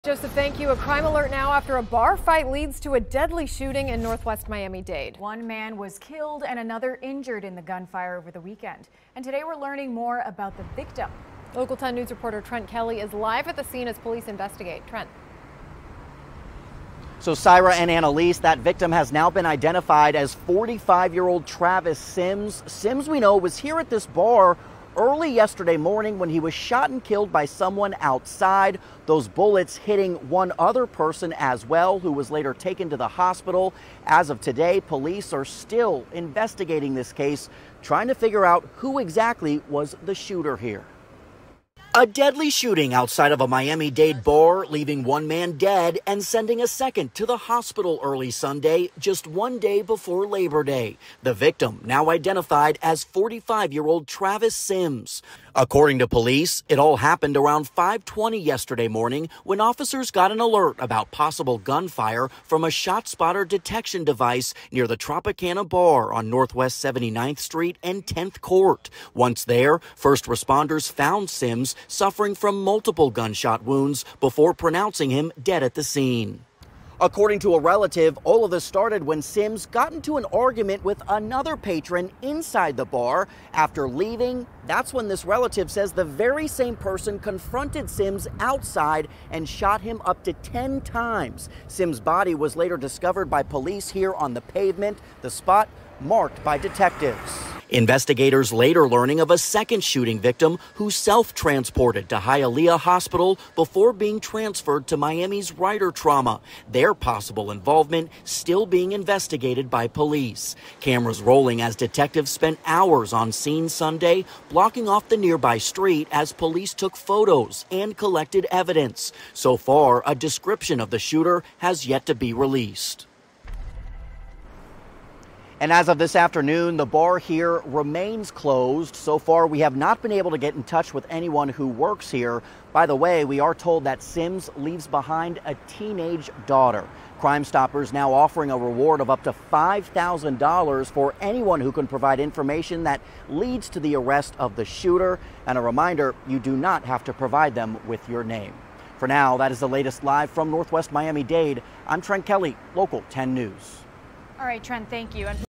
Victim identified after bar fight leads to deadly shooting in northwest Miami-Dade
A man died and another was injured during a shooting on Sunday morning outside of the Tropicana Bar & Lounge in northwestern Miami-Dade County, police said.